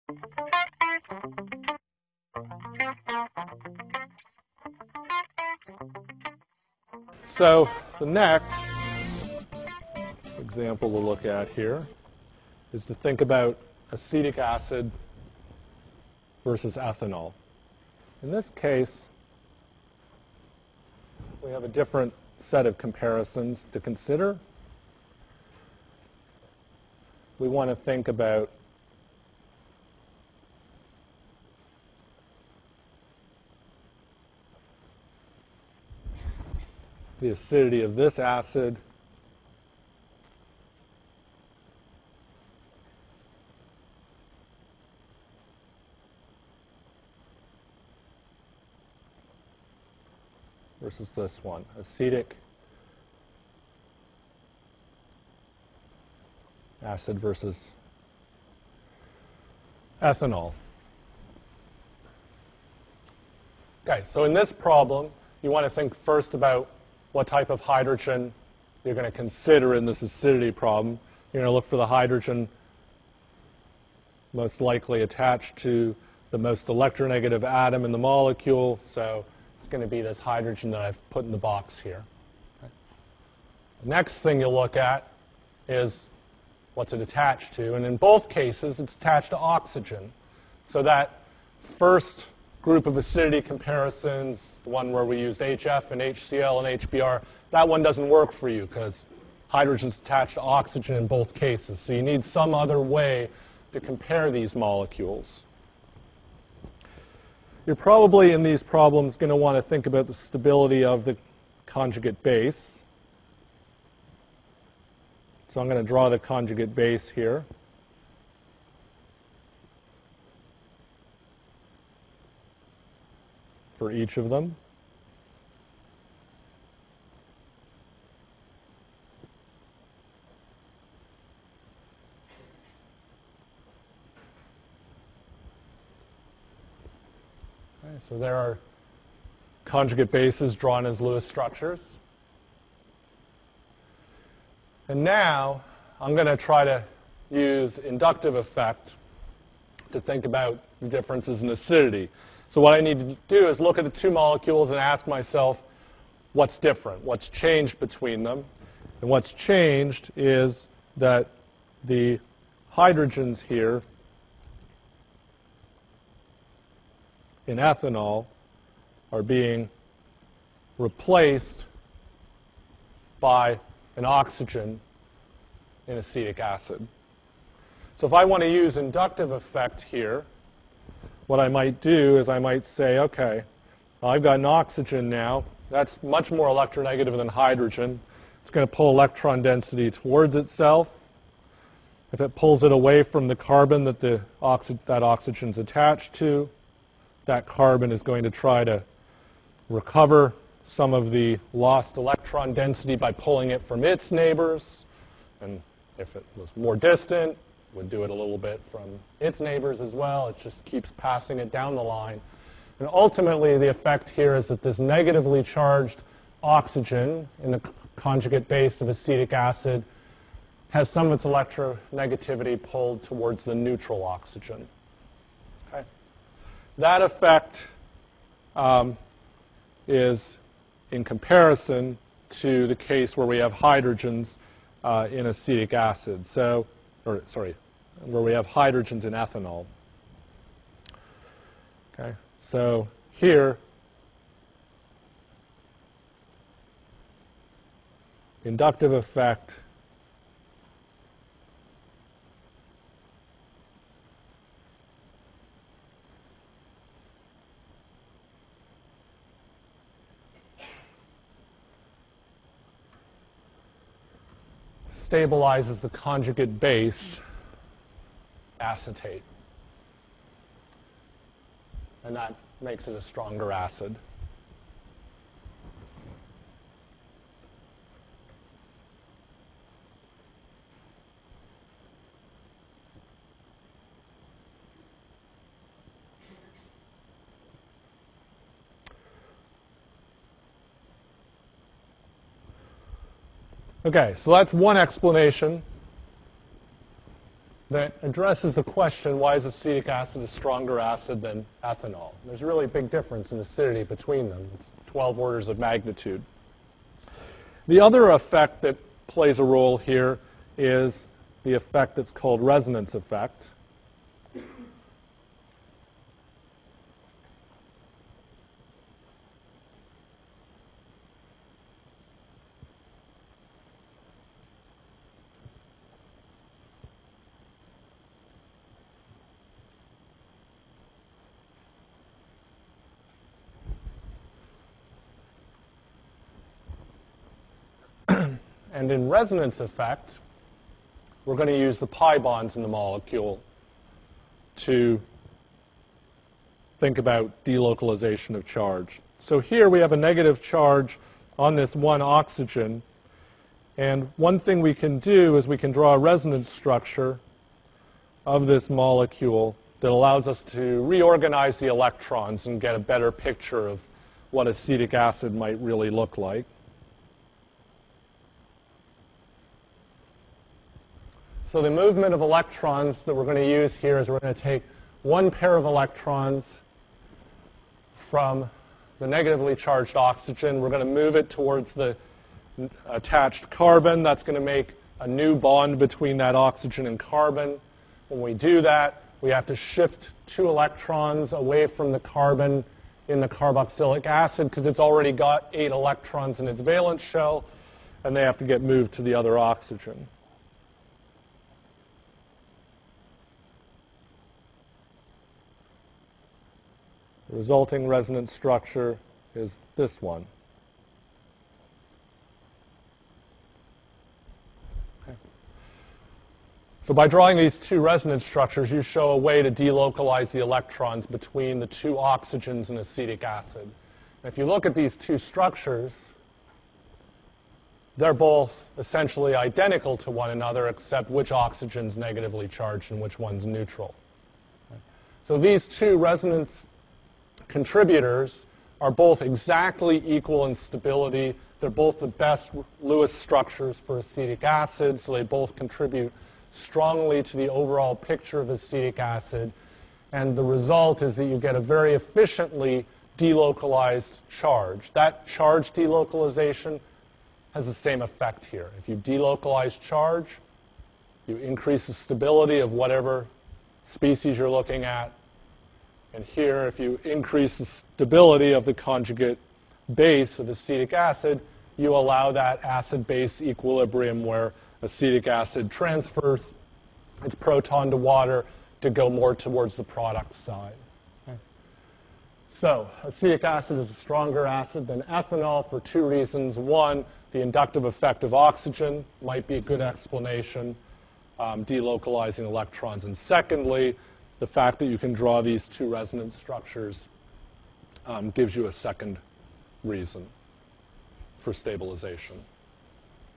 Lecture Problem: Inductive and Resonance Effects in Acidity
Why is acetic acid more acidic than ethanol?